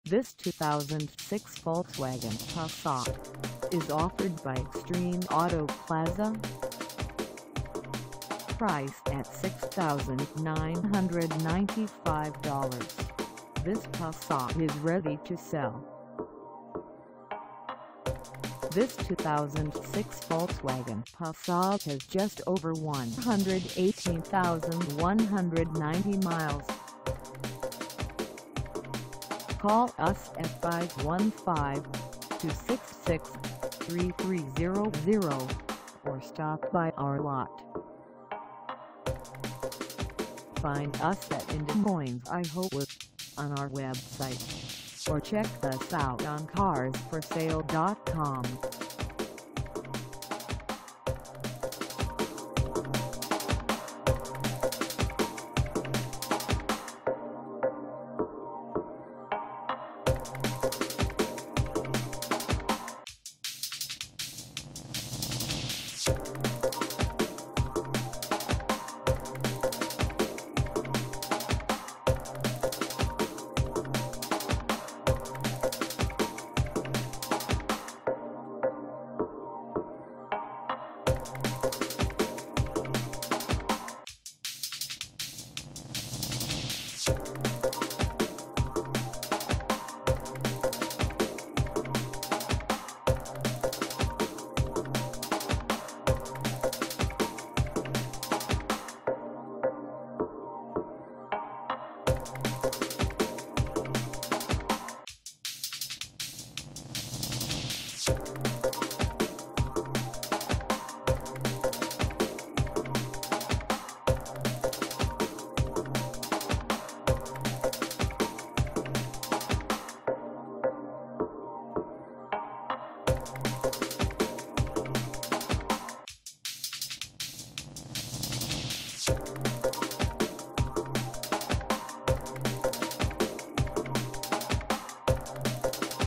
2006 Volkswagen Passat 3.6L V6 AUTO for sale in Des Moines,
This 2006 Volkswagen Passat 3.6L V6 AUTO is for sale in Des Moines, IA 50316 at Extreme Auto Plaza.
Engine: 6-Cylinder 3.6L
Transmission: Automatic
Color: Black
Mileage: 118190
Contact Extreme Auto Plaza  Des Moines, IA 50316 to test drive this 2006 Volkswagen Passat 3.6L V6 AUTO today.